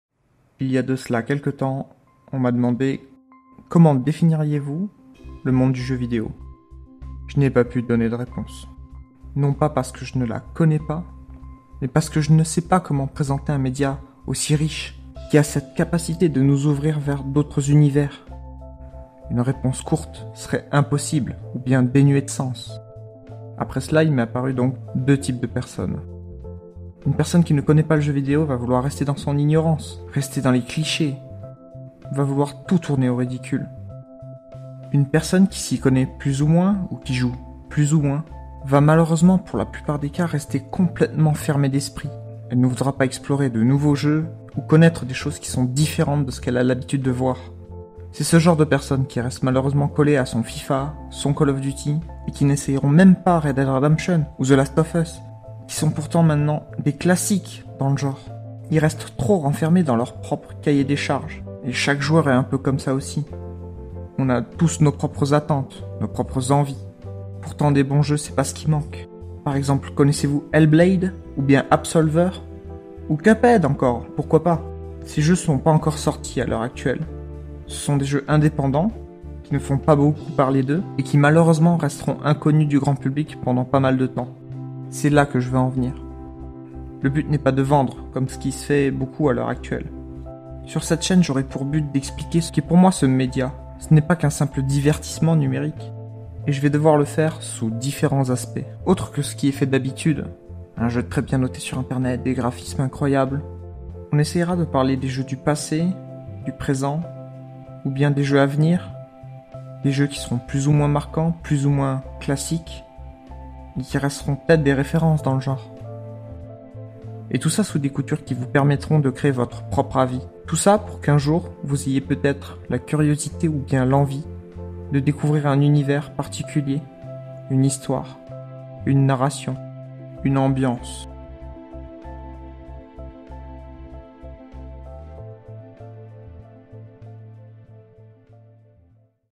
Pilote d'Explain Game Universe
Vidéo pilote d'une "émission" qui a pour but de parler du jeux vidéo autrement. Histoire de faire évoluer les mentalités, changer les avis sur ce média ou donner plus d'infos sur certains jeux.